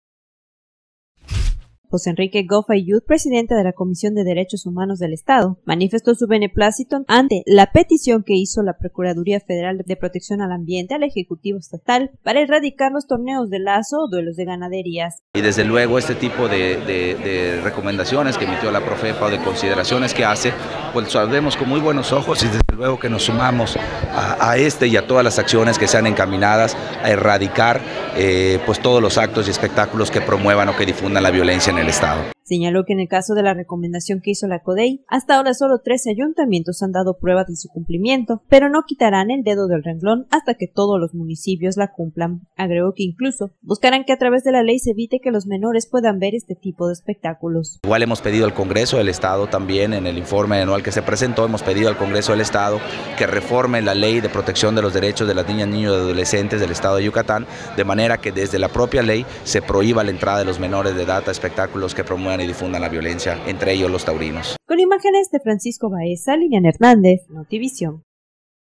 84860 CODHEY a favor de erradicar torneos de lazo
José Enrique Goff Ailloud, presidente de la Comisión de Derechos Humanos del Estado, manifestó su beneplácito ante la petición de la Procuraduría Federal de Protección al Ambiente al ejecutivo estatal para erradicar los “Torneos de Lazo” o “Duelo de Ganaderías”.